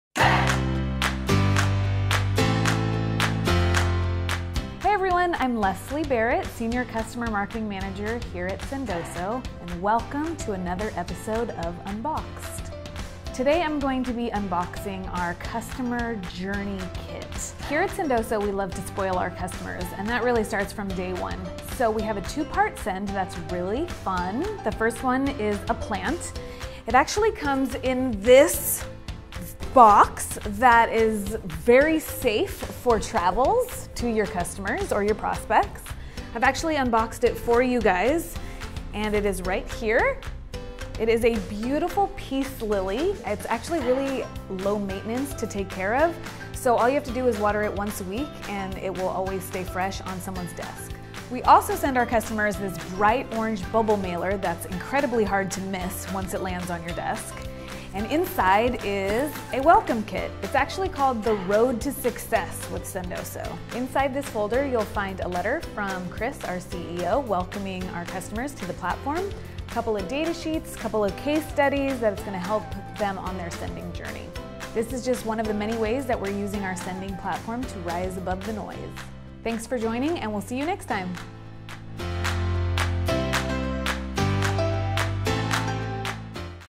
Unboxed: Sendoso Customer Welcome Plant & Onboarding Kit
Our newest episode of Unboxed, Sendoso Customer Marketing Manager Leslie Barrett shares two of the direct mail pieces we sent to our new customers: First, a welcome plant to inspire mutual growth. And second: A "Road to Success" welcome kit that helps customers get started with our platform!